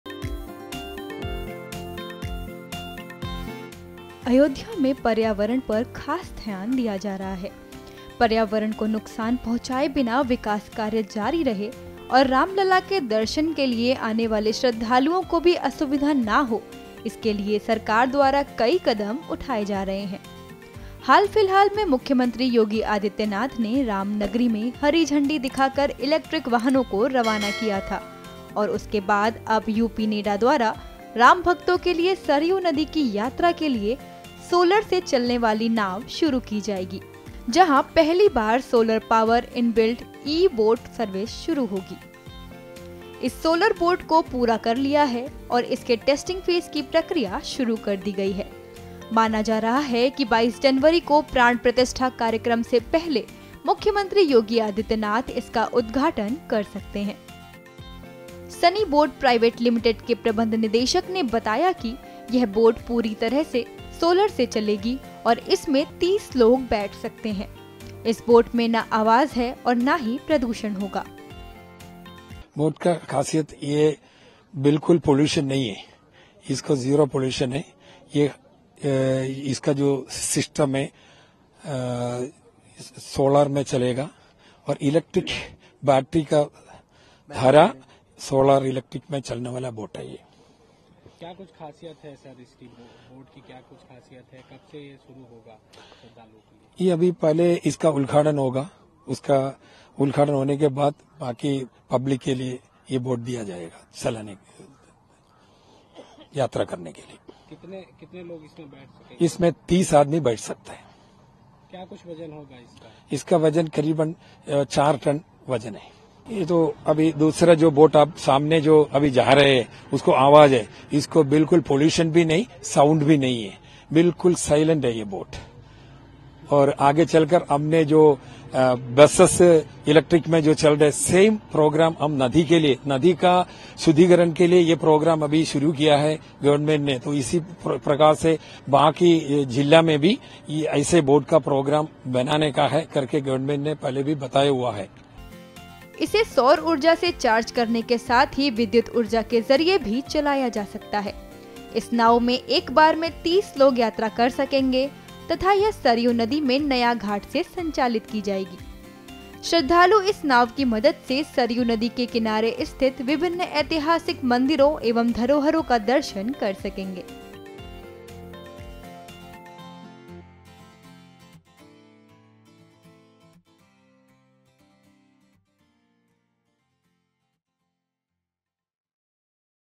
Ayodhya में पर्यावरण पर खास ध्यान, Sarayu नदी की यात्रा के लिए शुरू की Solar से चलने वाली नाव
अयोध्या में पर्यावरण पर खास ध्यान दिया जा रहा है। पर्यावरण को नुकसान पहुंचाए बिना विकास कार्य जारी रहे और राम लला के दर्शन के लिए आने वाले श्रद्धालुओं को भी असुविधा ना हो इसके लिए सरकार द्वारा कई कदम उठाये जा रहे हैं।